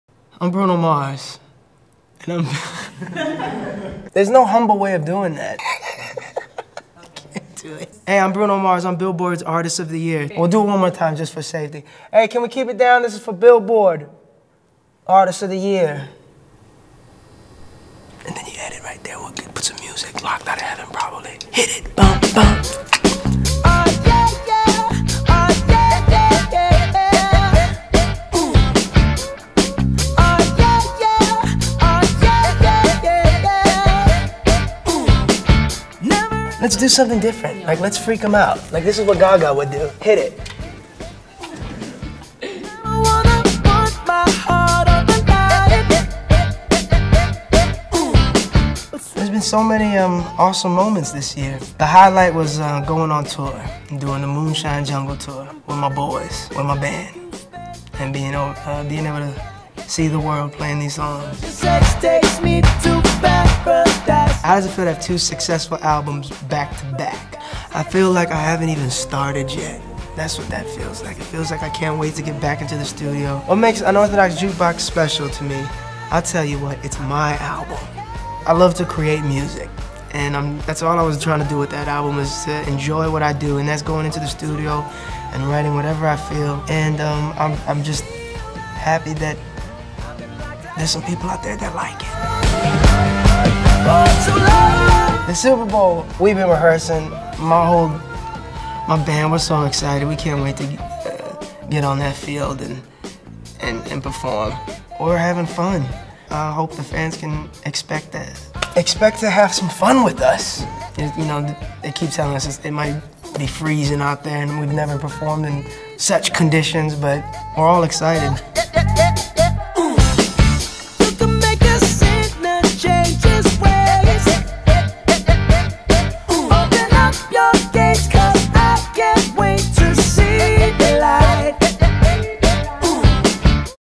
Bruno Mars: Billboard Artist of the Year 2013 - Cover Shoot + Q&A
How did Bruno Mars become Billboard's Artist of the Year? He grabbed hold of the album and single charts and never let go.
for more exclusive performances, interviews and in-depth coverage of the world's biggest music events.